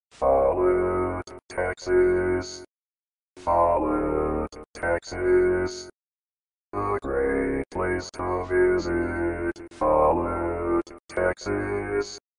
How to Say or Pronounce USA Cities — Follett, Texas
This video shows you how to say or pronounce Follett, Texas.

A computer said Follett, Texas. How would you say Follett, Texas?